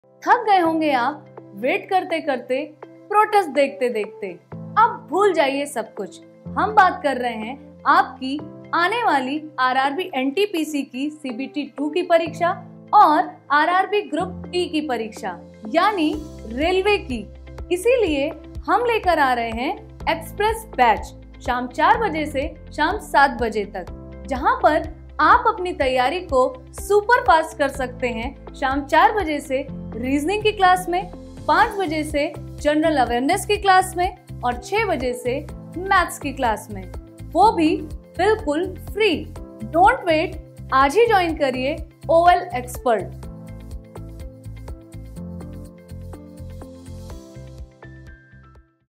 RRB NTPC CBT-2 & RRB Group-D Express Batch | Subscribe OLExpert For Free Classes & Study Material
Hello Students, OLExpert is coming up with RRB NTPC CBT-2 & RRB Group D Express Batch, where you will get to learn each & every topic of exam in depth & free of cost. RRB NTPC CBT-2 & RRB Group-D Express Batch is starting from 1st march 2022.